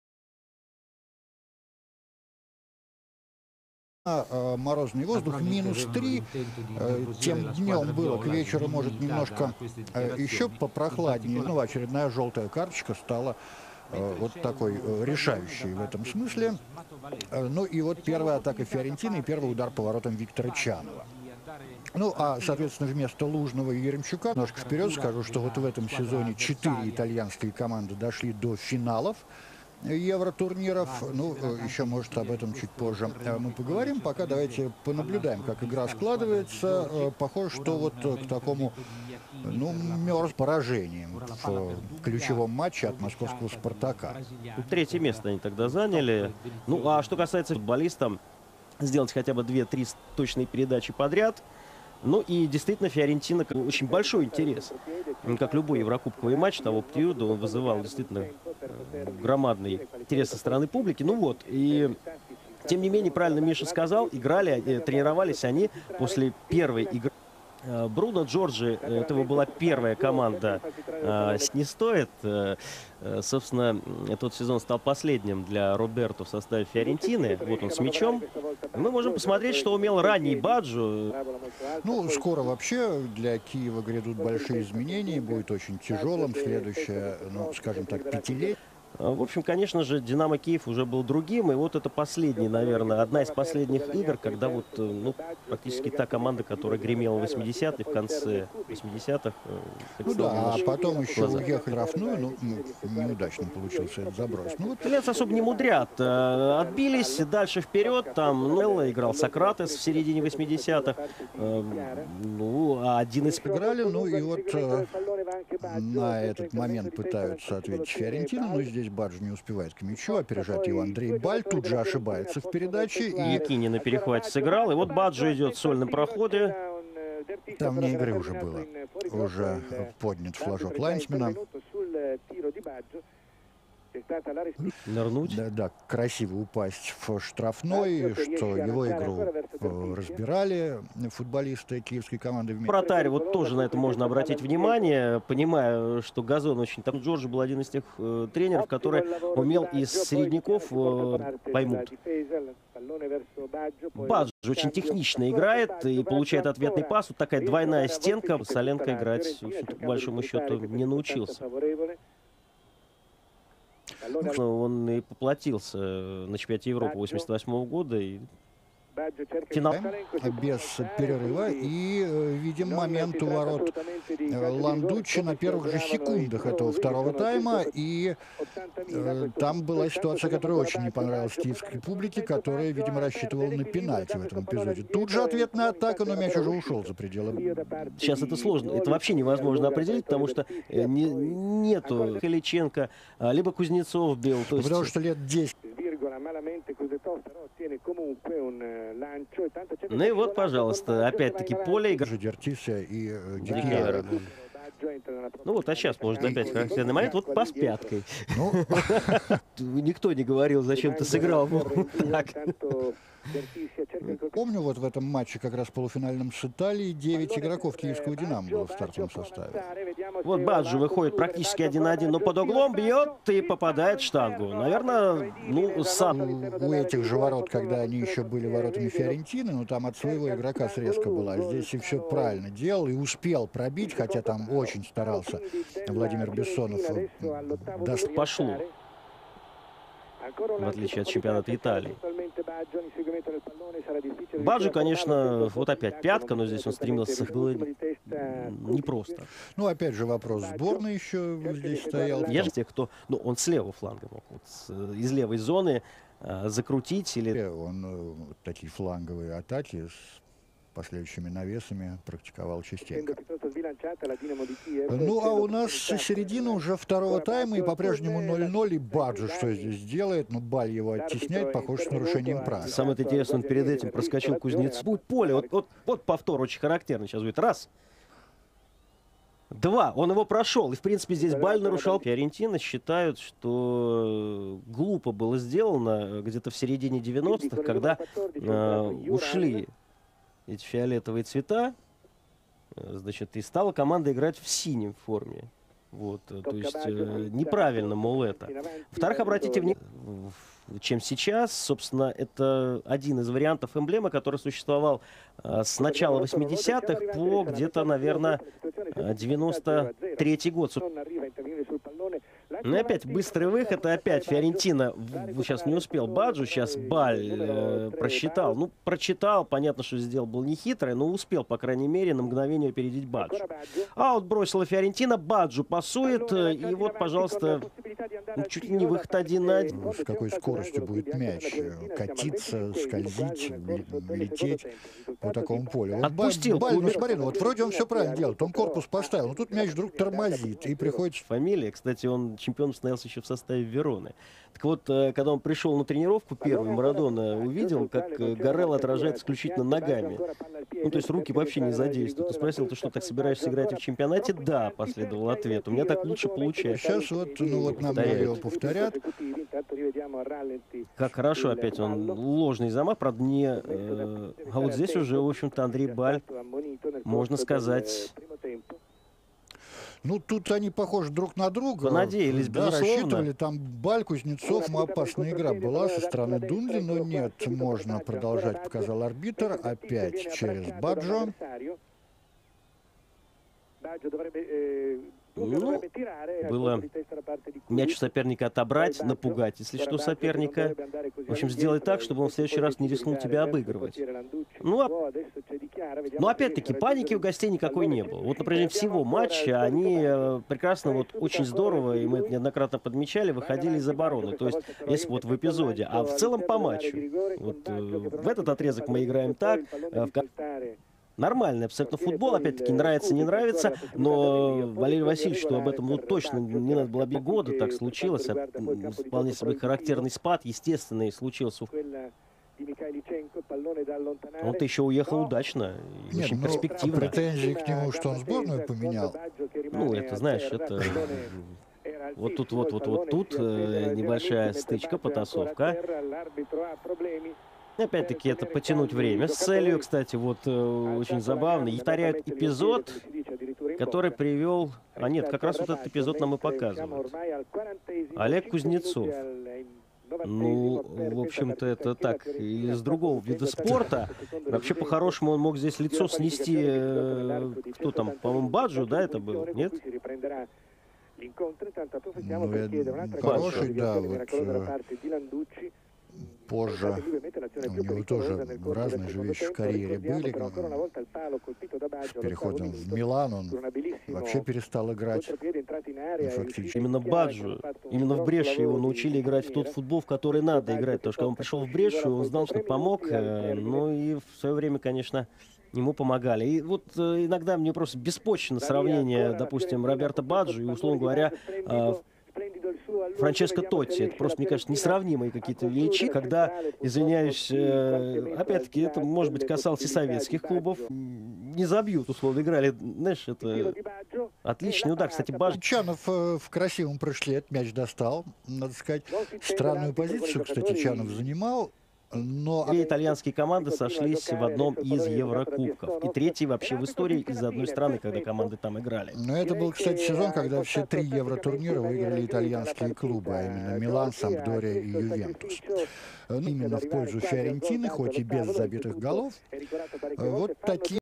Roberto Baggio vs Dynamo Kiev | 1989 UEFA Cup R16 2nd leg | All Touches & Actions
One of my favorite performances of Baggio personally, as he gives new meaning to the phrase 'gliding across the pitch gracefully'.

The match conditions were absolutely horrific to say the least. -3°C, icy pitch, the ball bouncing like a basketball and with the players sliding and slipping about and generally looking like statues stuck in ice, unsure about their footing.

Not Baggio though, as he went about waltzing around the Dynamo defense with feints, mazy dribbles and a brilliant turn of pace. Just phenomenal...

Dynamo Kyiv - Viktor Chanov, Oleg Kuznetsov, Sergiy Shmatovalenko, Sergiy Zayets, Volodymyr Bezsonov, Gennadiy Lytovchenko, Oleksiy Mykhaylychenko, Vasyl Rats, Andriy Bal, Oleg Protasov, Oleg Salenko

Manager - Valeriy Lobanovskyi

AC Fiorentina - Marco Landucci, Celeste Pin, Stefano Pioli, Alberto Di Chiara, Giuseppe Volpecina, Sergio Battistini, Dunga, Giuseppe Iachini, Lubos Kubik, Roberto Baggio, Óscar Dertycia

Manager - Bruno Giorgi